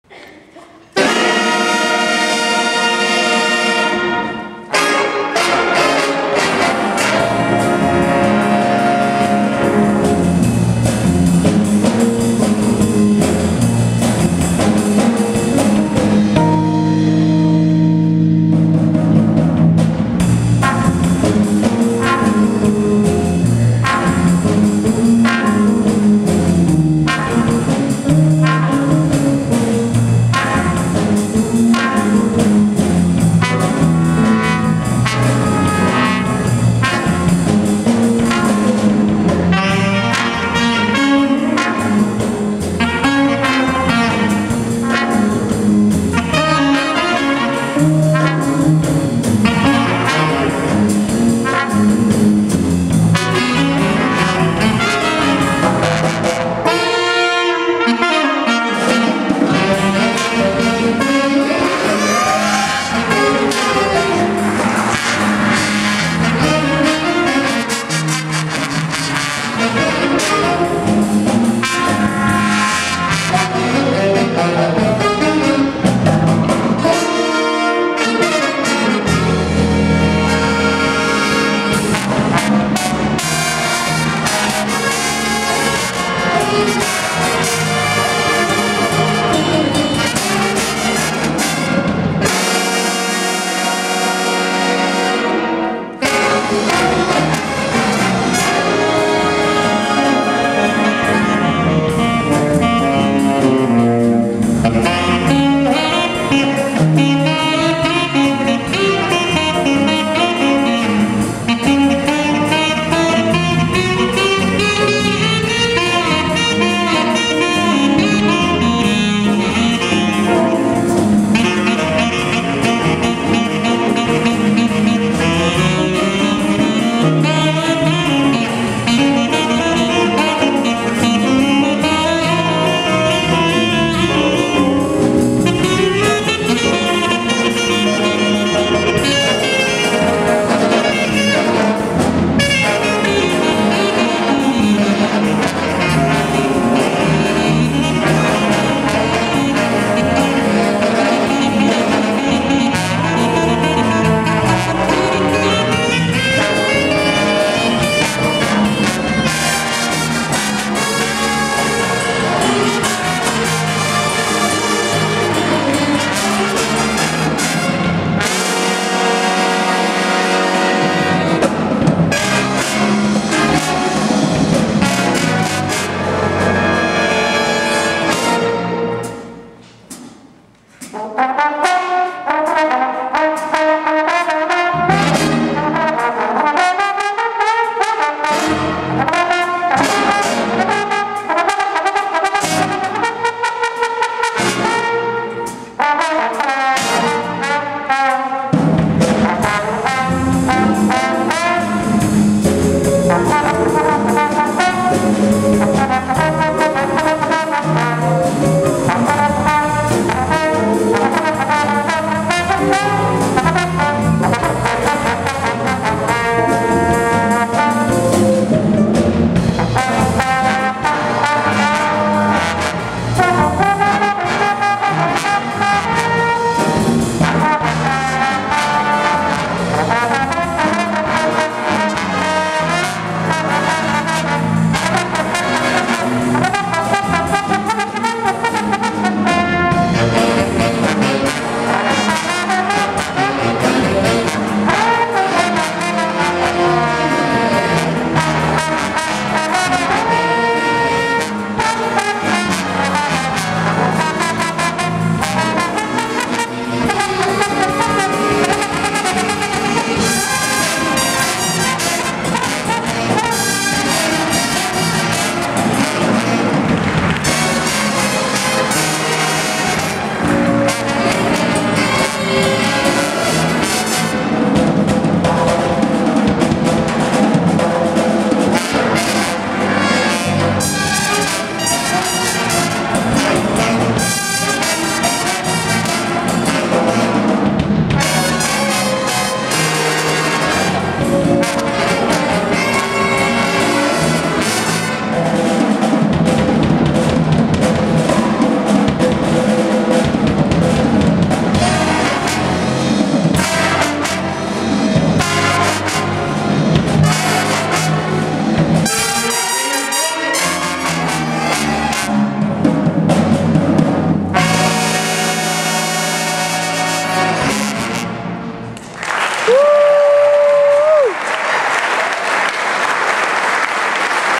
More Easton Area High School jazz ensemble at Freedom
Alec Redd, sax solo
Luke Farrell, trombone solo